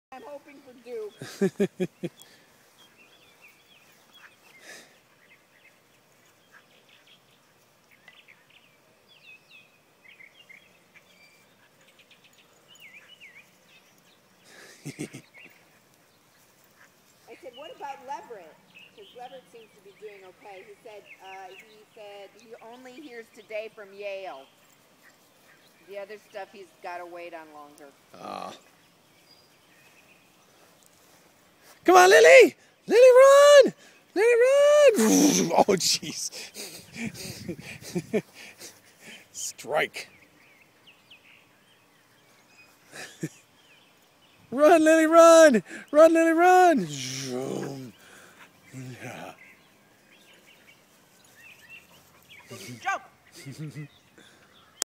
Lily and Eva playing
After only three months, look how big I'd gotten. Eva, my kitty friend, and I are playing, and my two dog friends, Toby (the black Shih-Tzu) and Trevor (the white Coca-Poo) get in on the game too. What fun we have running around the back yard!